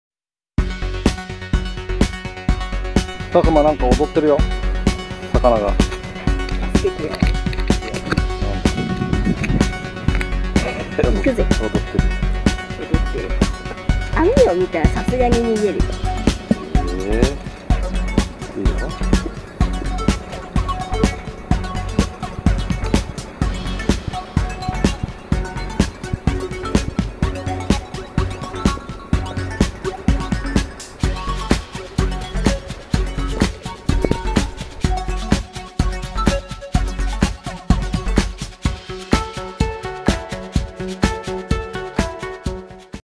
高田純次ダンスか？【イシダイの動画です】
海を見ていたら・・・

クルクル回るイシダイが・・・